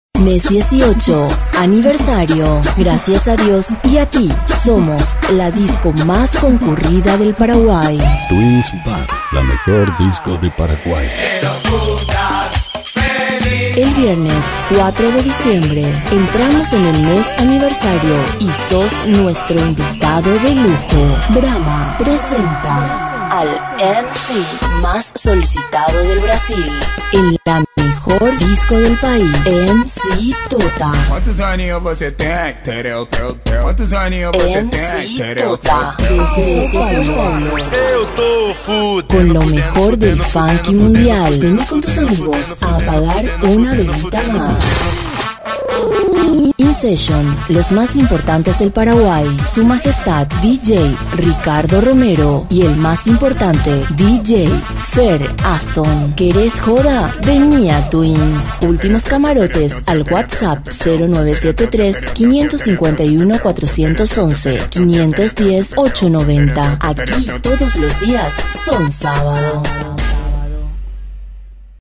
Twins Bar Paraguay MC TOTA 04 DIC
--MES 18 ANIVERSARIO-- Gracias a Dios & a Ti somos La Disco Mas Concurrida del Paraguay El Viernes 04 de Diciembre Entramos en el Mes Aniversario y sos nuestro Invitado de Lujo BRAHMA PRESENTA al Mc mas Solicitado del Brasil en la Mejor Disco del Pais **MC TOTA** Desde Sao Paulo Con lo Mejor del Funk Mundial Veni con tus Amigos a apagar una Velita mas